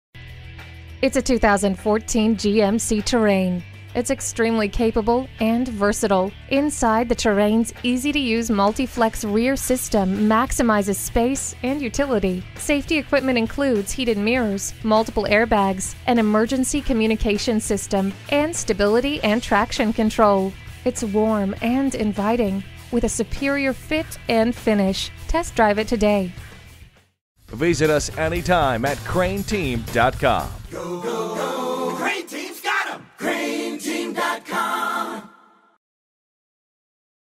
2014 GMC Terrain Conway Little Rock, AR 4GT4461 - SOLD
Year: 2014
Make: GMC
Model: Terrain
Trim: FWD SLE1
Engine: 2.4L DOHC 4-Cyl SIDI Spark Ignition DI Engine
Transmission: Automatic
Color: Summit White
Interior: Light Titanium
Stock #: 4GT4461

Address:
1003 North Museum Road